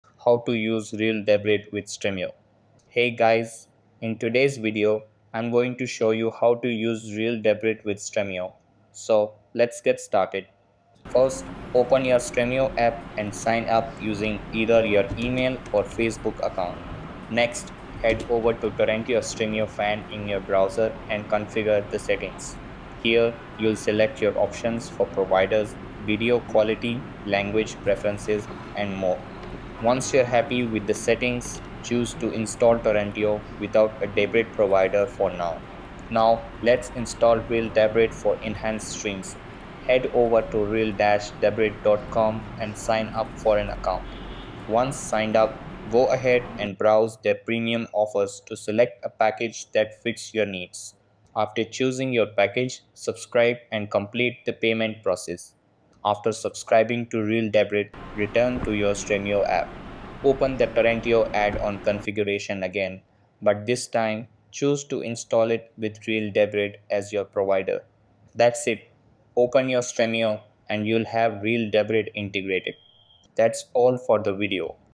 HOW TO USE REAL DEBRID WITH STREMIO 2025! (FULL GUIDE)
Unlock a world of high-quality streaming with Real Debrid and Stremio! In this comprehensive guide, we’ll walk you through the steps to integrate Real Debrid with Stremio for a seamless streaming experience. Learn how to set up your Real Debrid account, link it to Stremio, and maximize your access to premium links and content. Say goodbye to buffering and hello to smooth streaming in 2025!